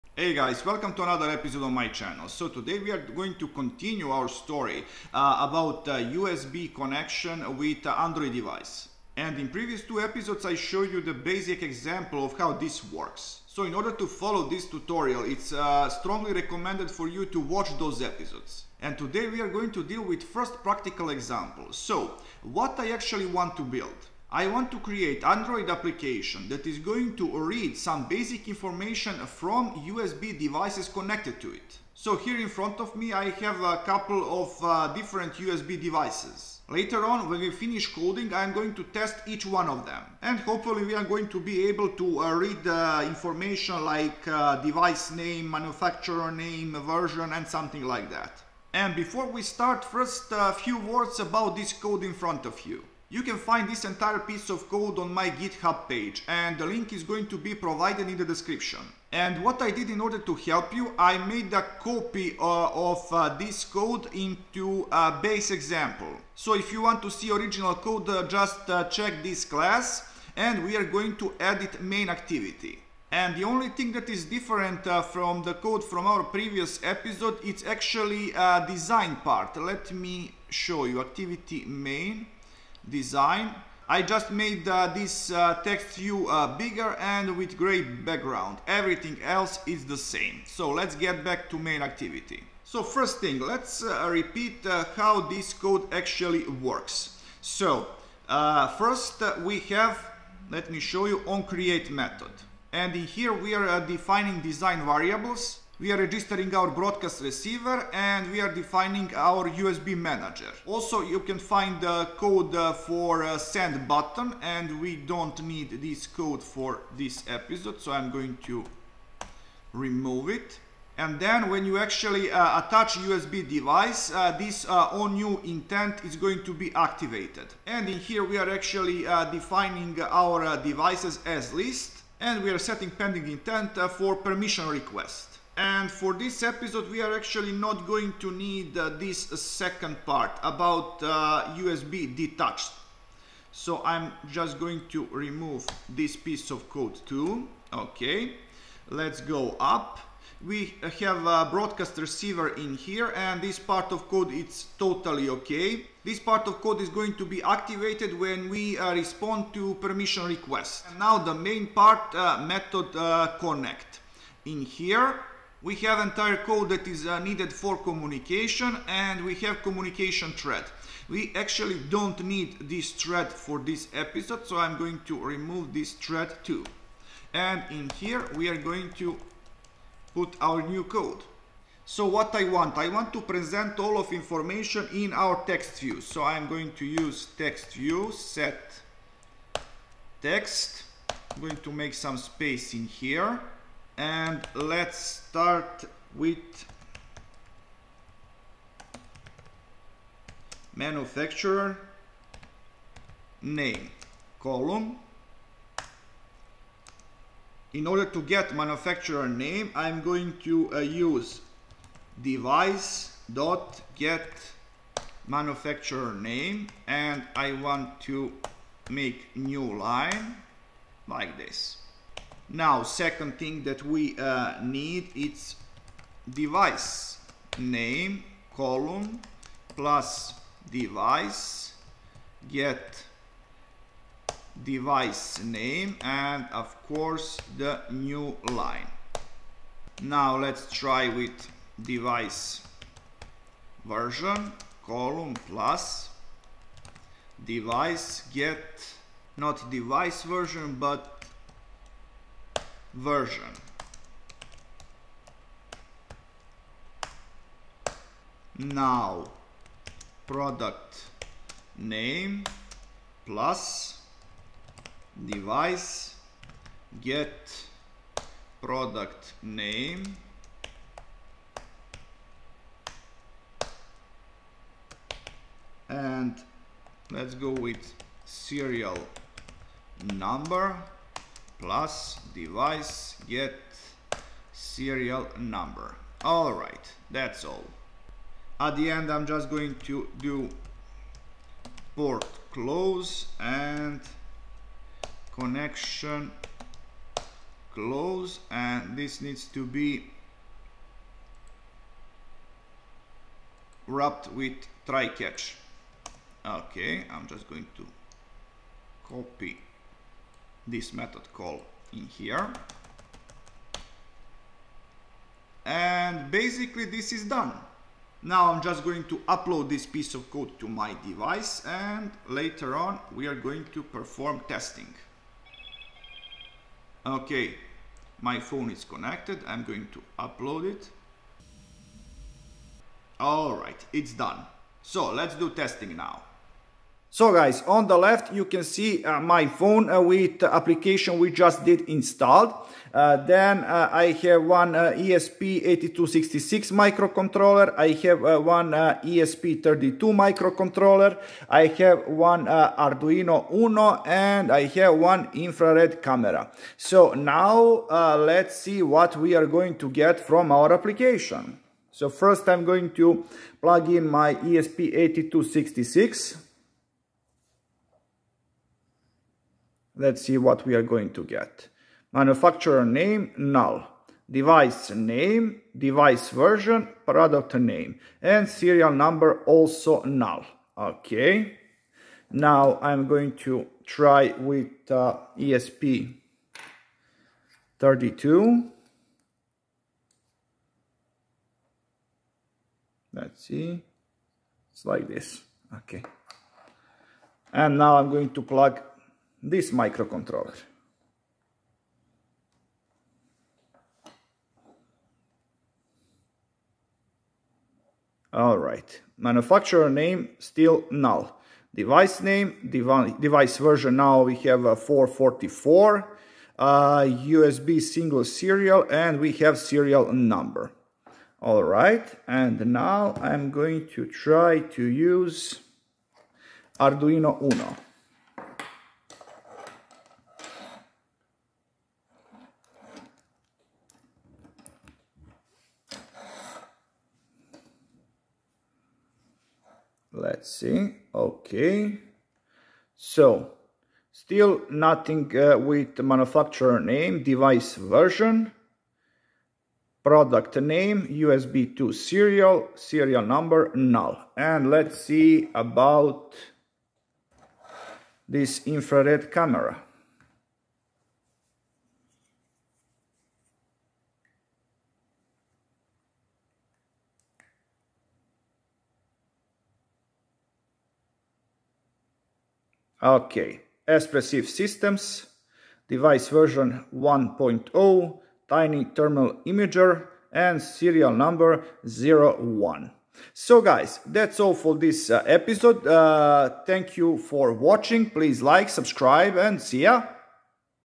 ESP32, ESP8266 and Android Communication over USB, read USB device meta data
In this episode of ESP32, ESP8266 and Android programming, we are going to discuss reading of metadata from connected USB device.